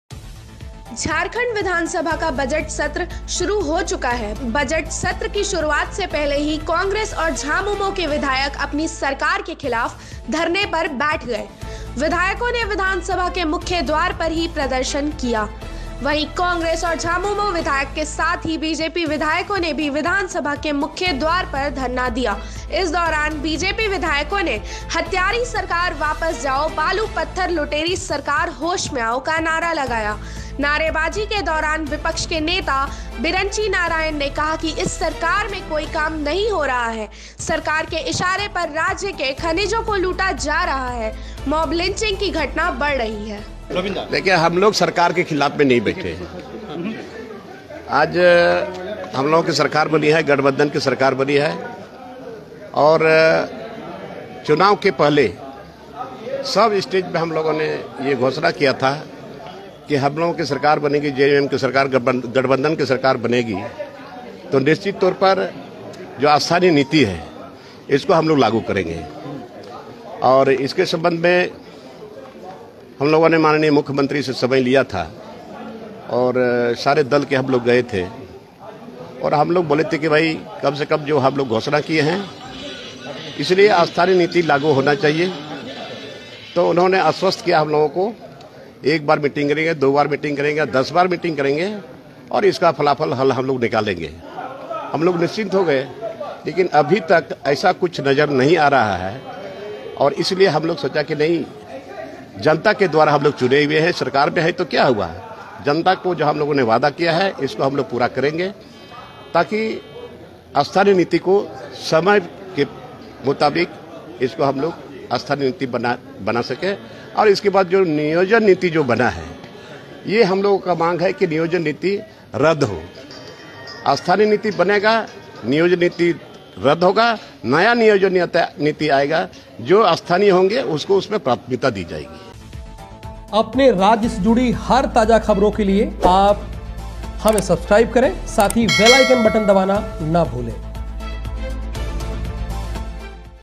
स्थानीय नीति के मांग को लेकर अपने ही सरकार के खिलाफ धरना पर बैठे झामुमो, कांग्रेस विधायक
अपनी ही सरकार के खिलाफ धरना पर बैठे झामुमो , कांग्रेस विधायक